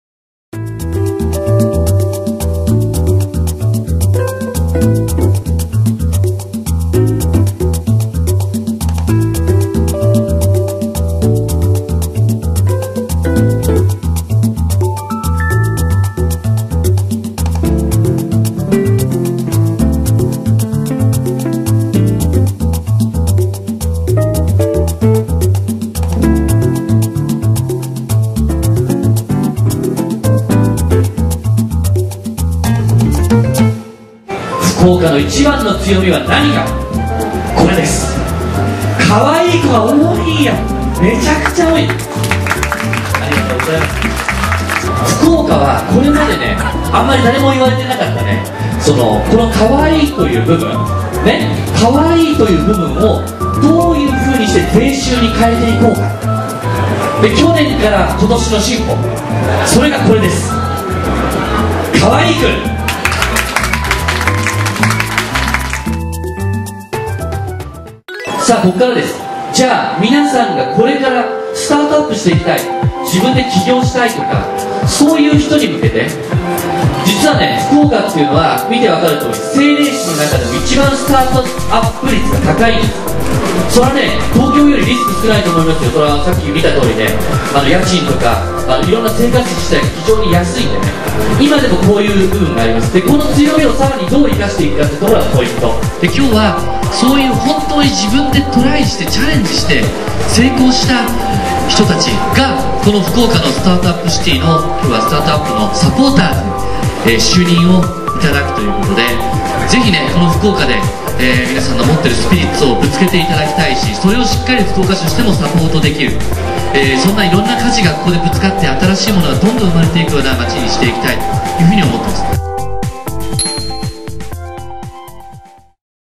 高島福岡市長が「スタートアップ都市」事業を発表！／明星和楽(QBC)
9月8・9日に中洲ゲイツビルで行われたテクノロジー＆クリエイティブのフェスティバル「明星和楽」。ここから、福岡・日本、そしてアジアを盛り上げる、未来のスターやクリエイターの発掘・育成を目指します。
9月8日には、福岡市長の高島宗一郎氏がステージに登場。福岡市を「クリエイティブ・エンターテインメント都市」にするため、「スタートアップ都市・ふくおか」事業を設立することを発表しました。(続きはQBCで)